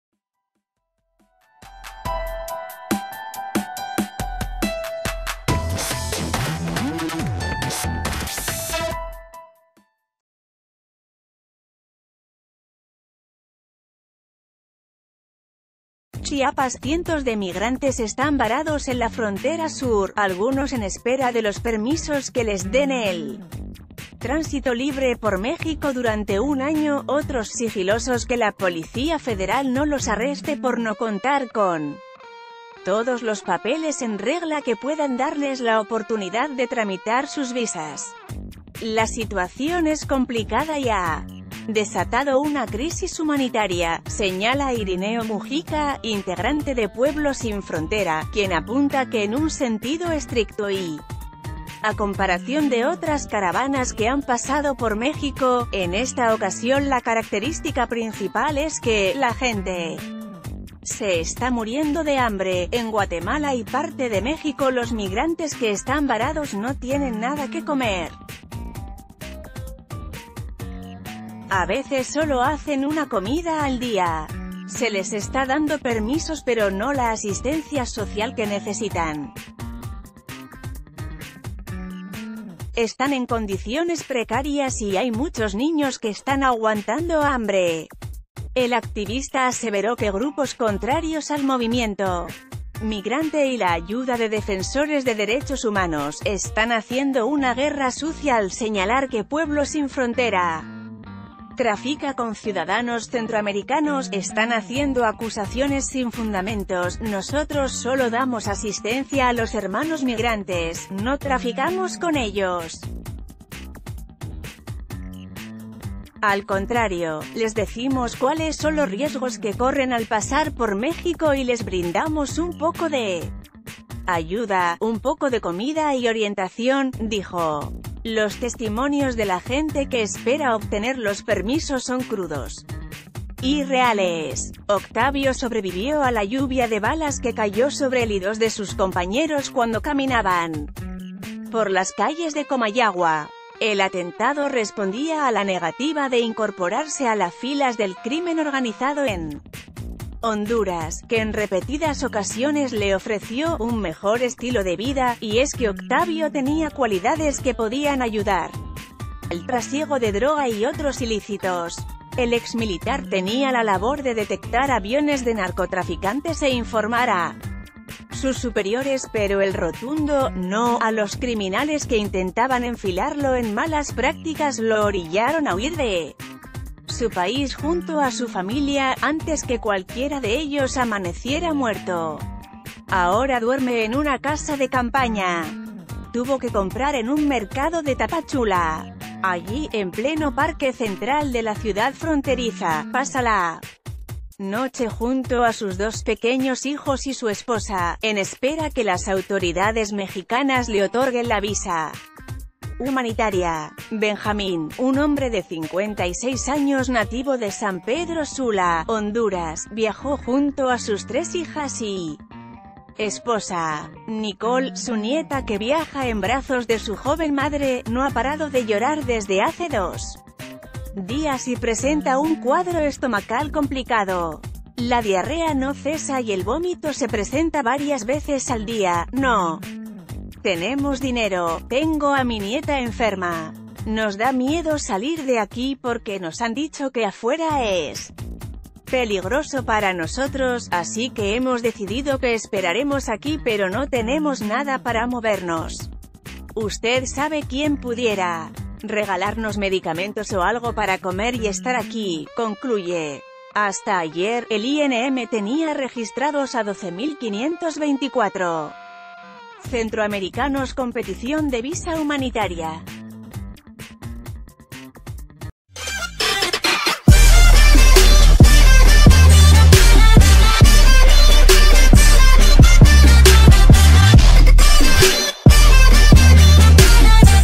Frontera sur vive una crisis humanitaria
Chiapas.- Cientos de migrantes están varados en la frontera sur, algunos en espera de los permisos que les den el tránsito libre por México durante un año, otros sigilosos que la Policía Federal no los arreste por no contar con todos los papeles en regla que puedan darles la oportunidad de tramitar ...